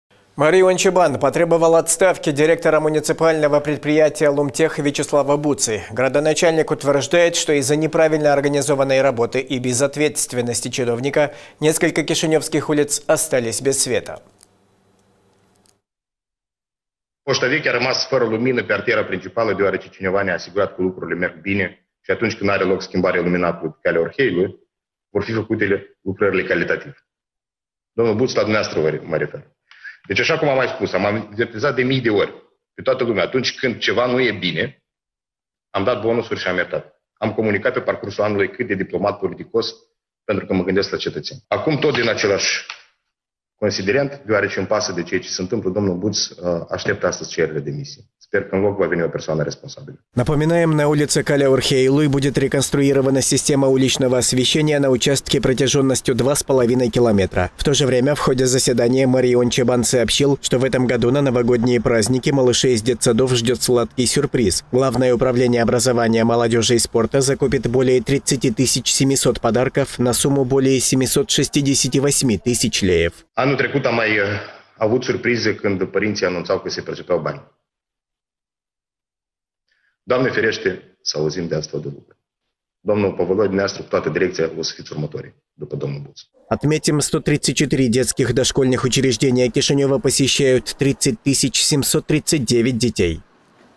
"LUMTEH" ОСТАЛСЯ БЕЗ ДИРЕКТОРА
Мэр Ион Чебан потребовал отставки директора муниципального предприятия Lumteh Вячеслава Буцы. Градоначальник утверждает, что из-за неправильно организованной работы и безответственности чиновника, несколько кишинёвских улиц остались без света.